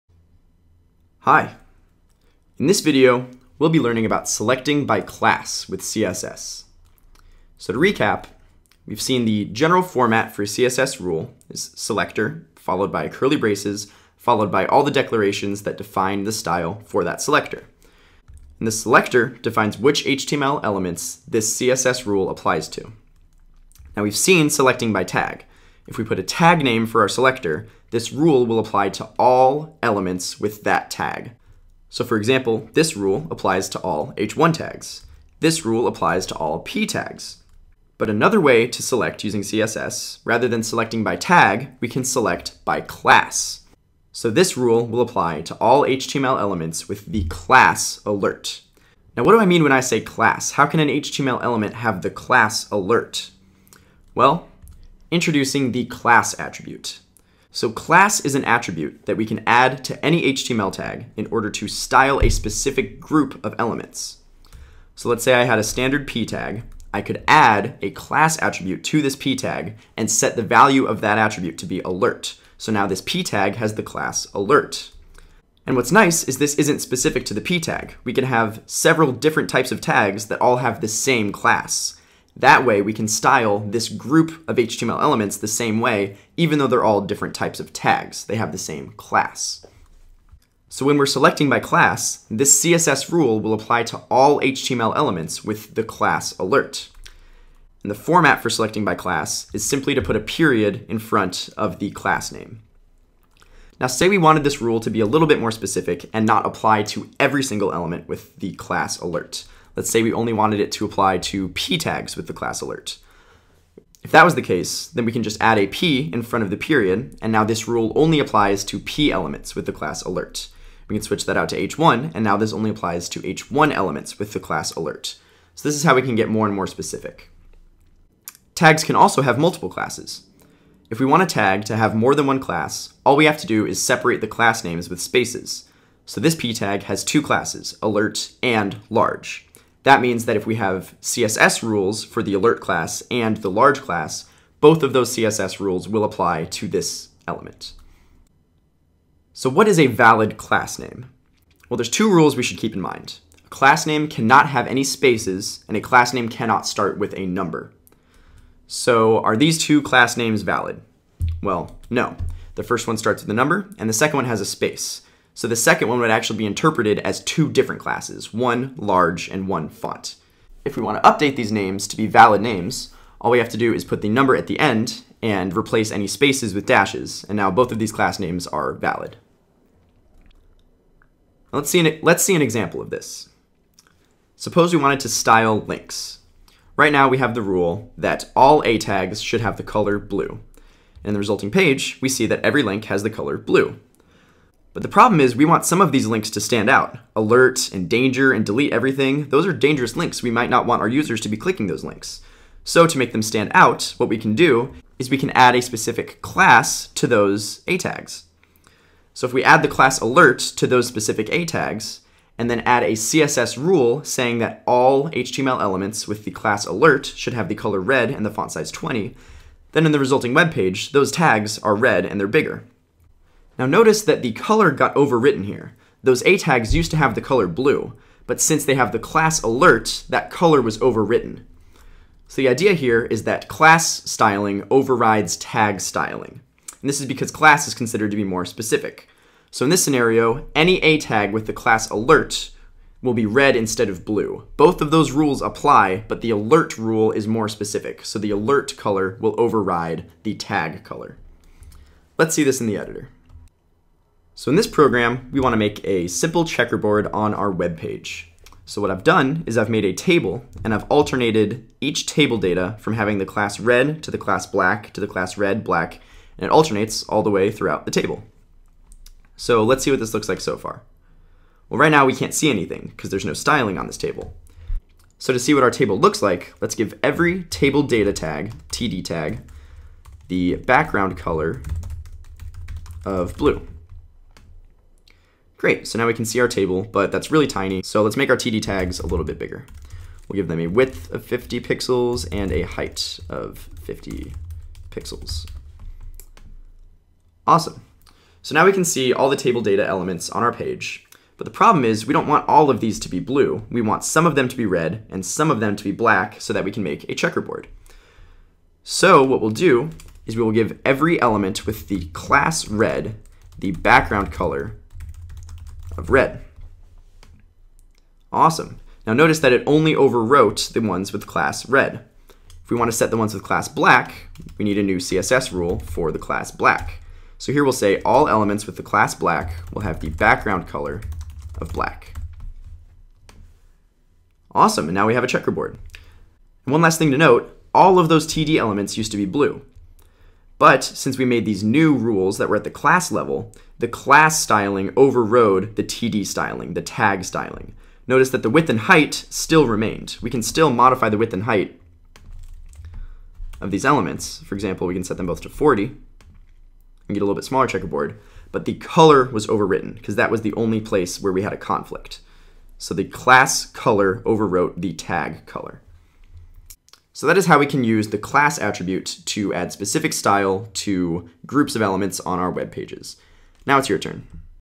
CSS Select by Class
We learn about using the class attribute to select groups of elements by class and style them with CSS.